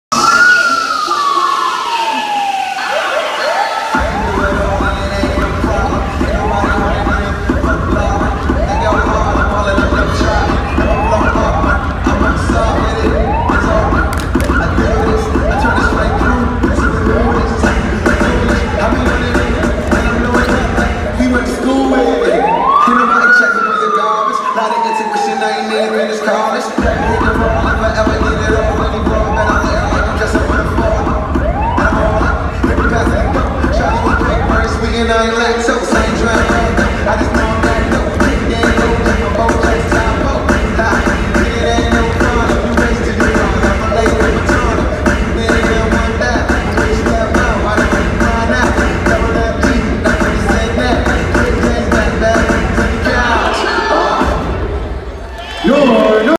North Texas Dancers: Mean Green Basketball vs UTA
Date: December 8, 2018
Style: Hip-Hop
Choreography: Kennedy W